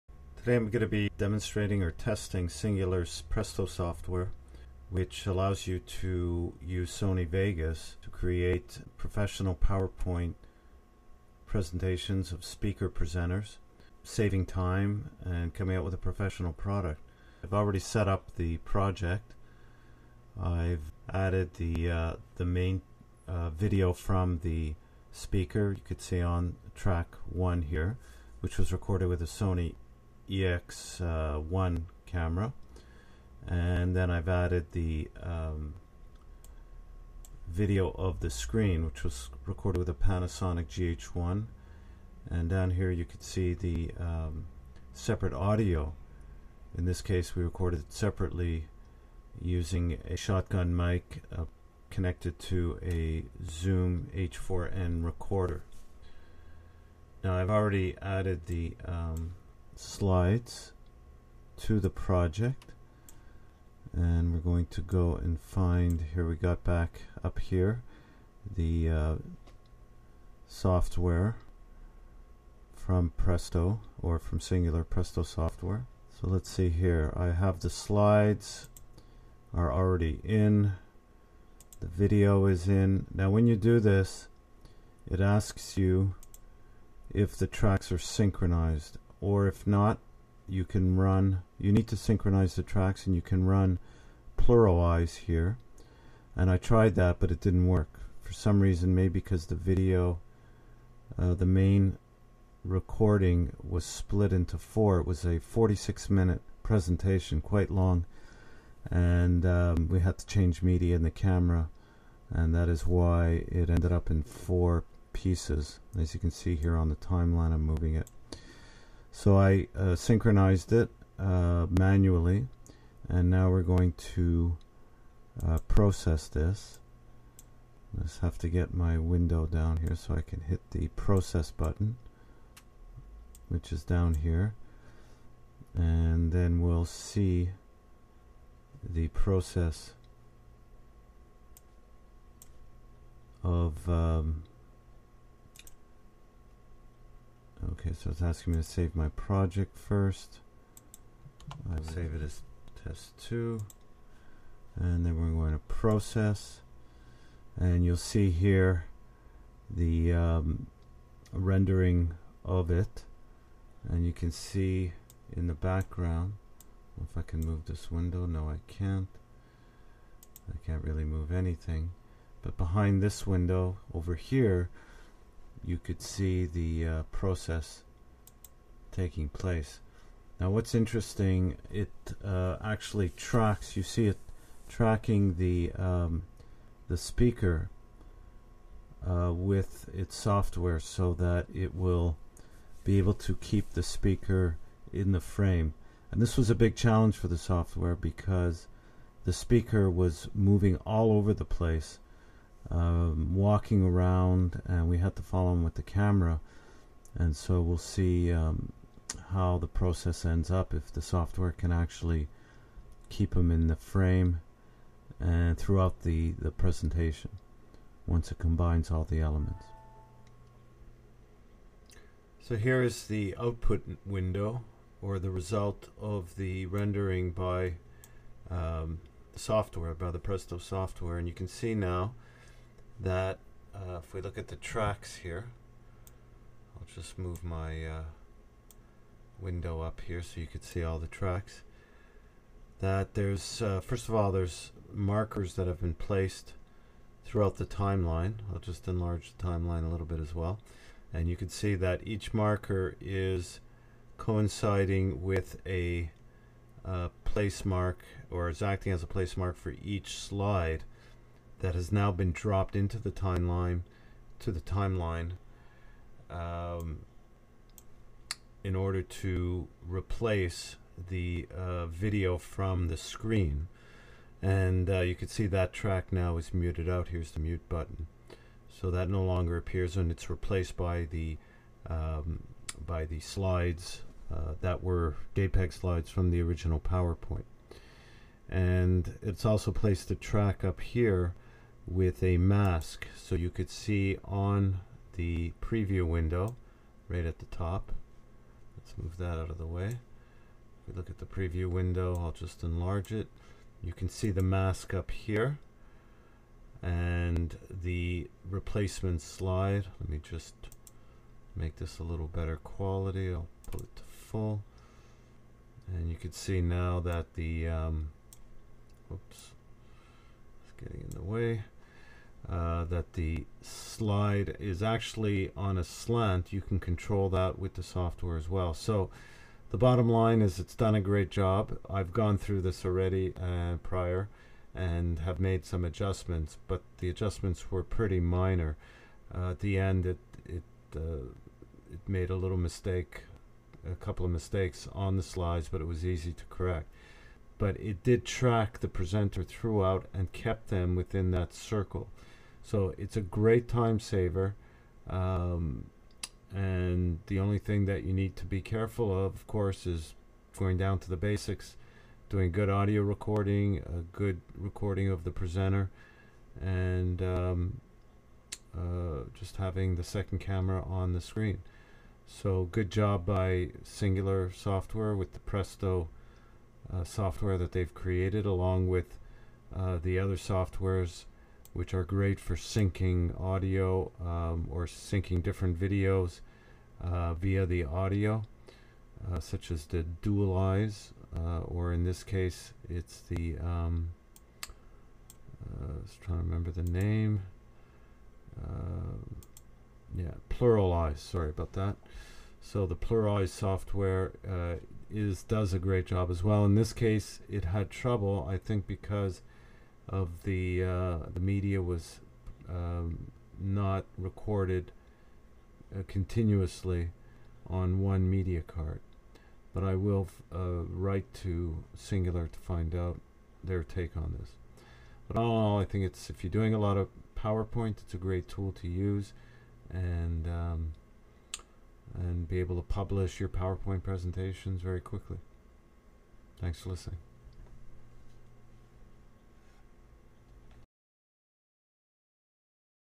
Singular Presto review.mp4
This is a video review of Presto by Singular software. I have been testing the software on a few projects for recording PowerPoint presentations and this video shows the results.